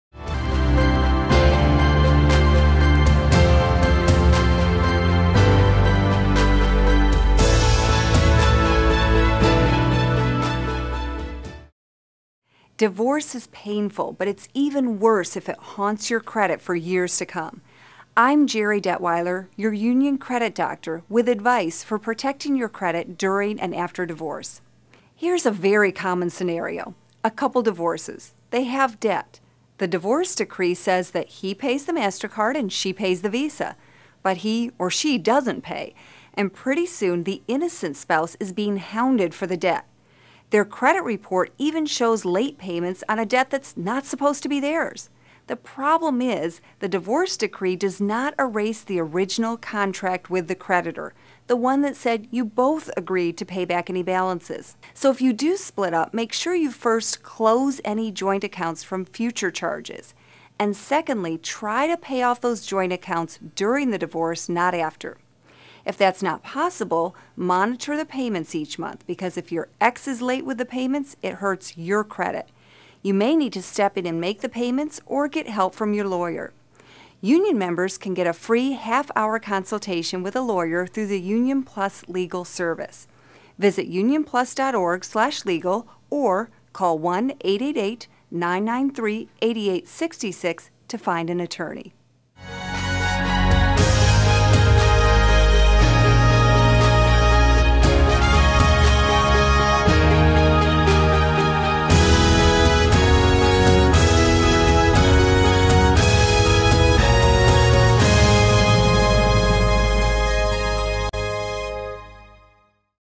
Divorce and Your Credit Score
A divorce could impact your credit for years to come. It is important to protect your credit during and after divorce.

For example, if a husband and wife have joint MasterCard and Visa accounts and the divorce settlement stipulates that the husband pay off the MasterCard account and the wife will be responsible for the Visa credit balance, but one of them doesn't make the payments, both parties will be penalized on their credit reports.

Tips for handling credit when filing for a divorce:
First, close joint accounts to prevent further charges
Second, pay off joint accounts during the divorce, not after
Third, monitor the monthly bills to make sure payments are made
Fourth, get help from a lawyer if needed

Learn about the legal recourse you have to protect your credit in the event of a divorce. Union members can get a free half hour legal consultation with an attorney through the Union Plus Legal Service. or call to find an attorney.